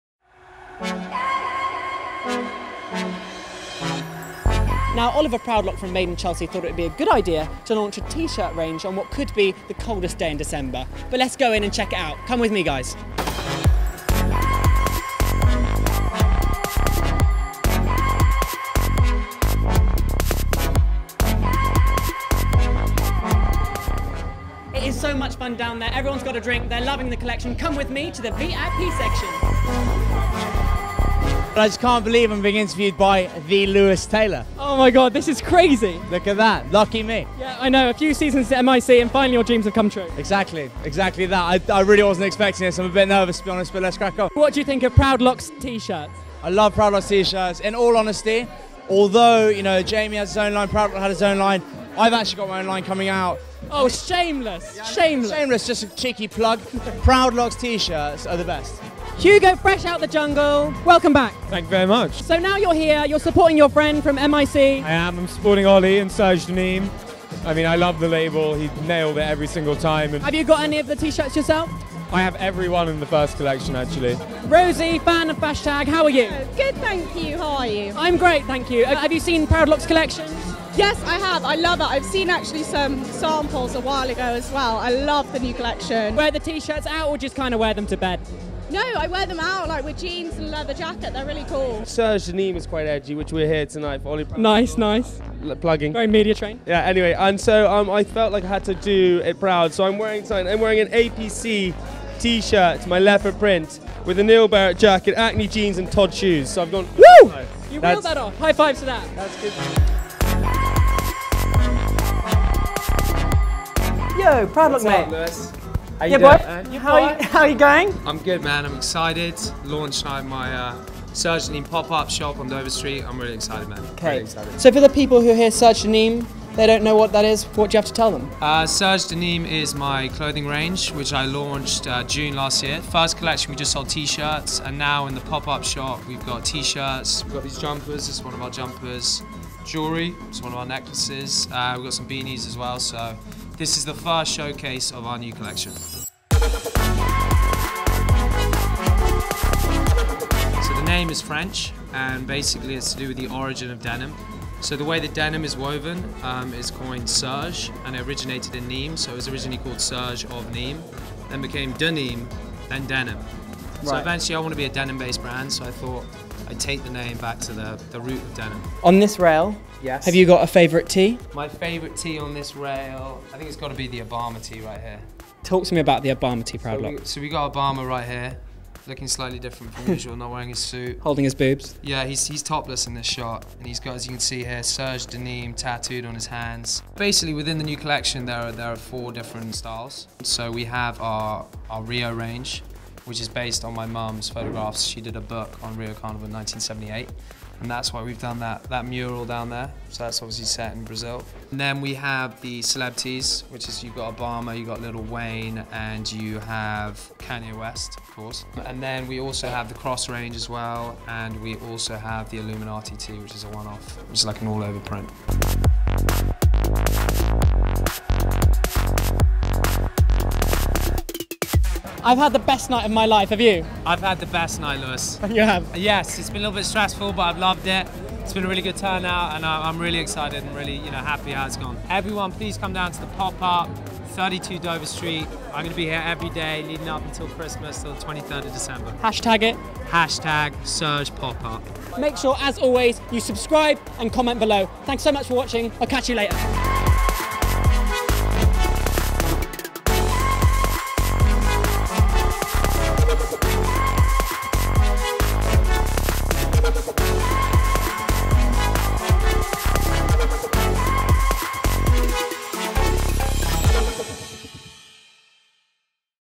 Oliver Proudlock of Made In Chelsea - Serge DeNimes Launch Party | FASHTAG
Made In Chelsea's Oliver Proudlock launches a pop-up store for his Serge DeNimes clothing line. We sent Lewis Taylor down to mingle with the MIC set.

Spotted on the night: Spencer Matthews, Hugo Taylor, Rosie Fortescue, Jamie Laing

The Serge DeNimes pop-up shop is open right now until the 23rd of December, downstairs at Wolf & Badger, 32 Dover Street, Mayfair, London.

The collection includes the celeb range: Obama, Lil Wayne and Kanye West; the Rio range and the cross range.